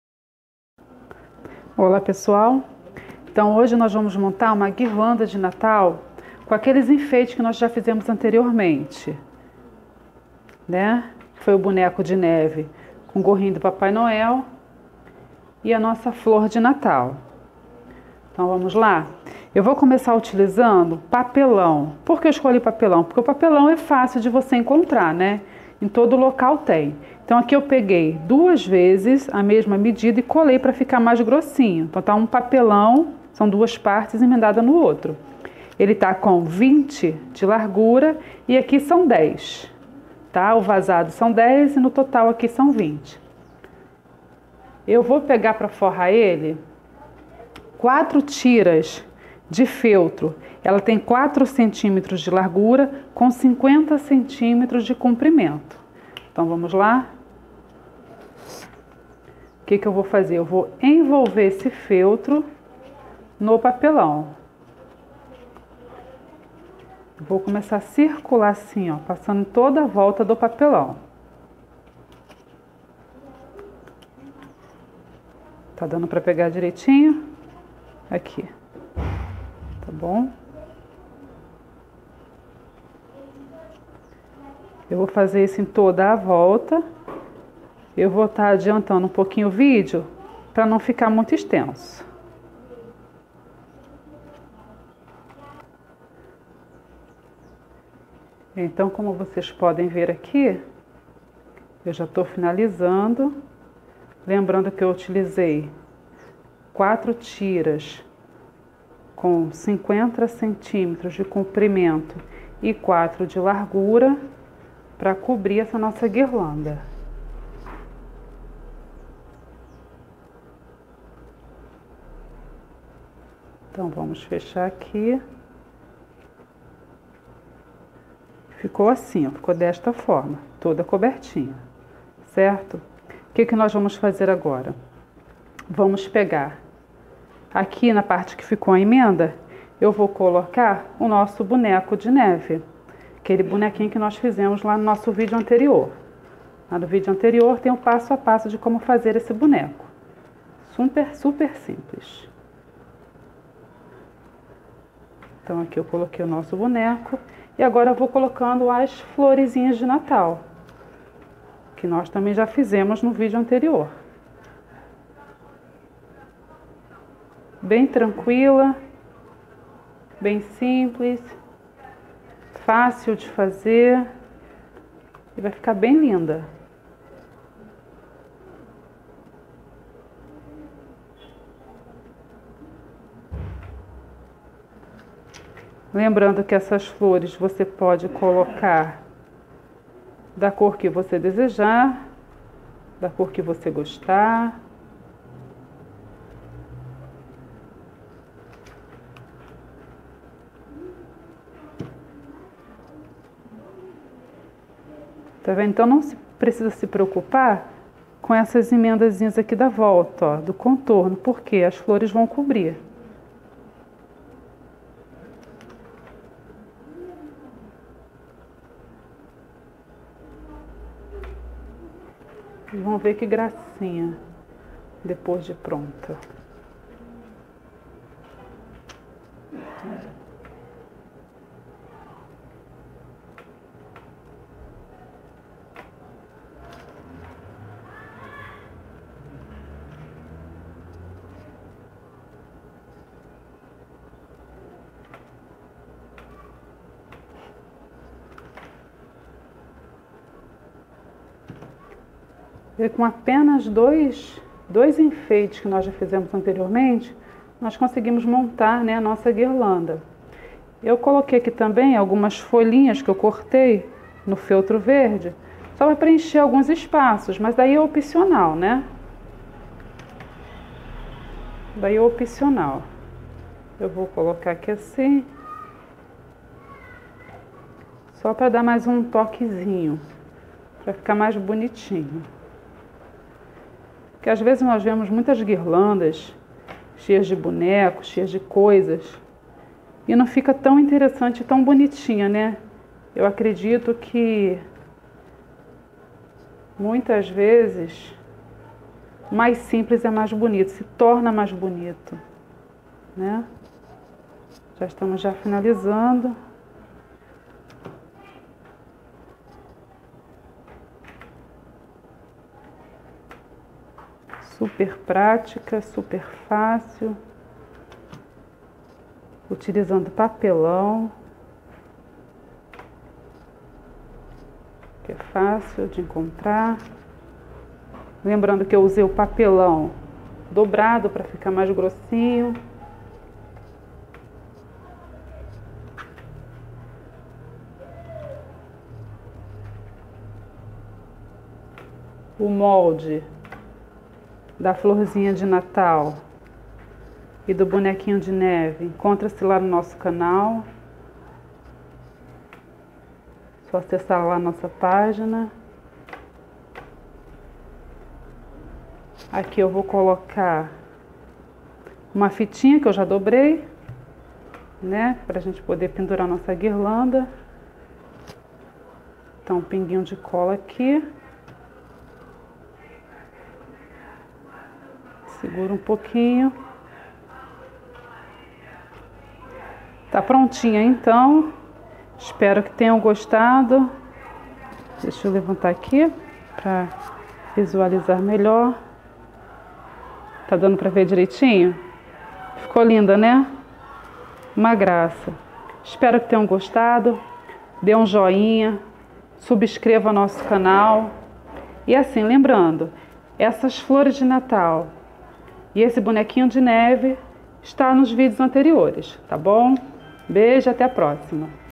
Como fazer uma guirlanda de Natal em Feltro Passo a Passo!
Esta Guirlanda de Natal é um Lindo enfeite Natalino que traz um charme especial a sua decoração de Natal
De uma forma bem simples e rápida para personalizar seu Natal !